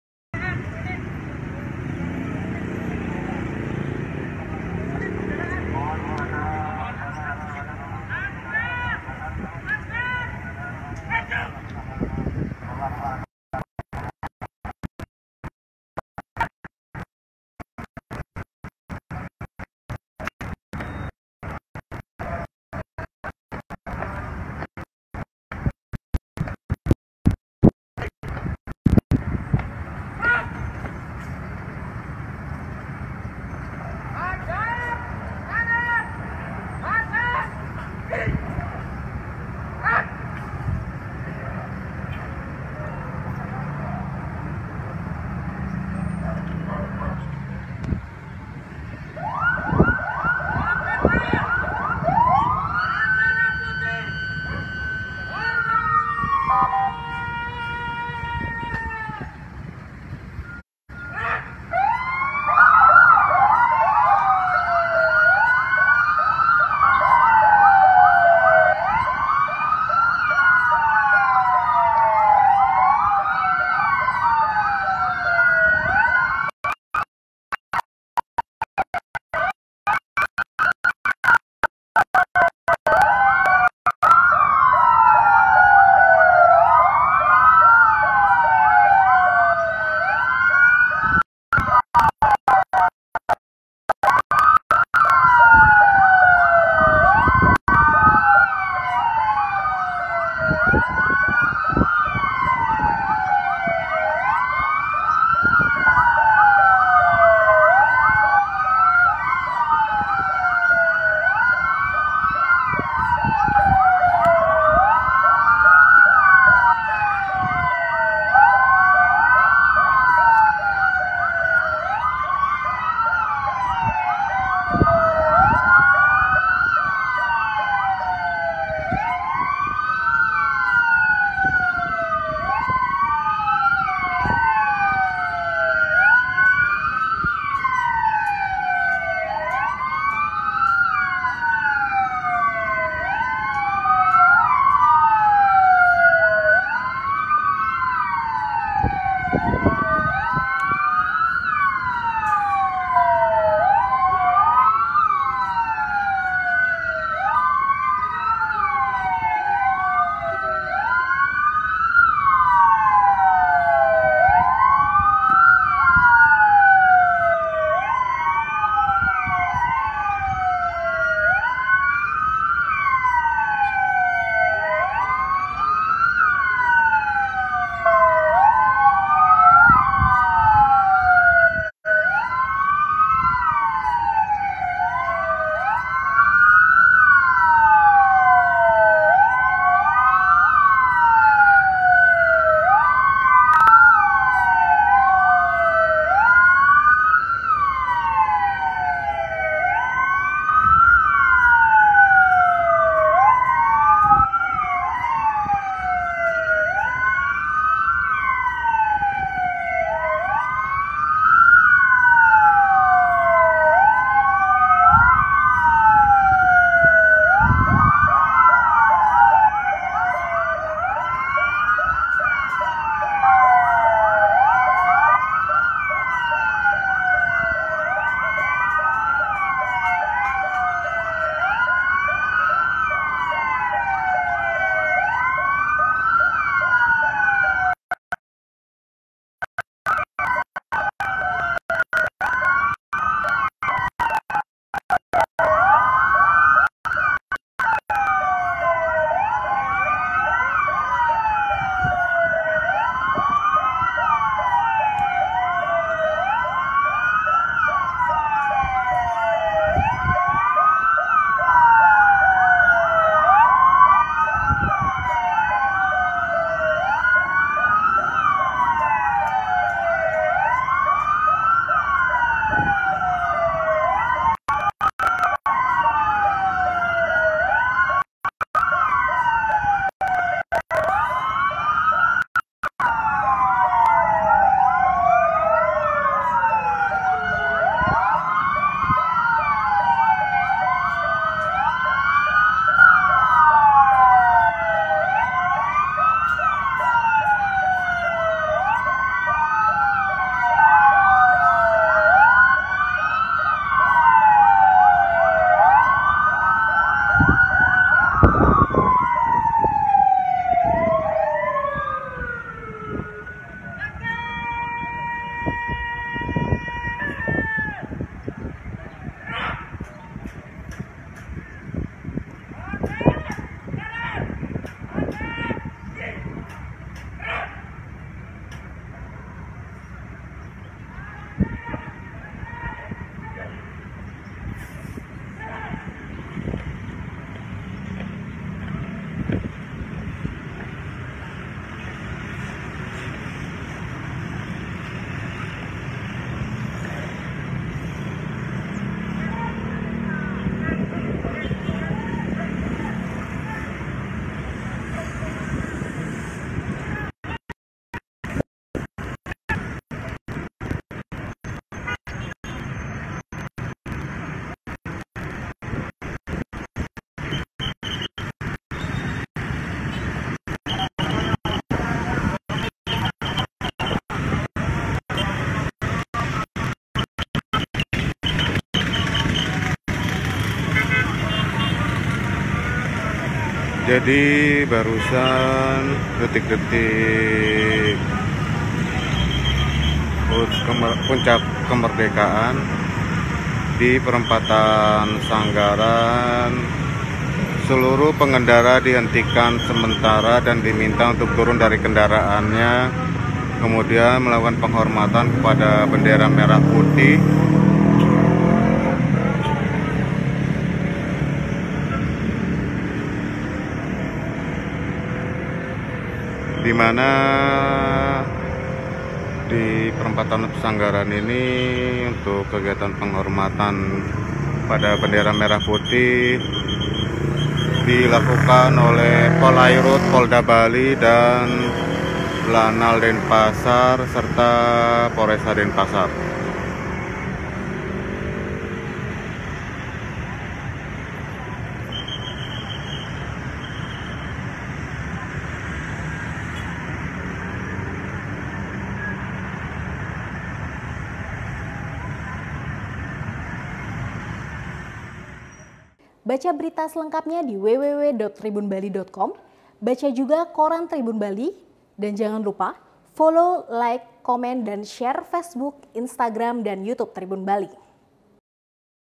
Pengendara di Perempatan Pesanggaran Denpasar Lakukan Penghormatan Pada Merah Putih
Perempatan Pesanggaran Denpasar tampak tidak seperti biasanya siang ini, sekira pukul 10.50 Wita terlihat sejumlah personel dari Kepolisian dan TNI khususnya Angkatan Laut berjaga disana.
Tak lama terlihat personel membawa tiang dengan bendera merah putih berada di perempatan.
Jelang pukul 11.17 Wita seluruh kendaraan bermotor dihentikan dan diminta turun dari kendaraannya dan melakukan penghormatan sejenak kepada bendera merah putih.

Tribun Bali/Apriadi Gunawan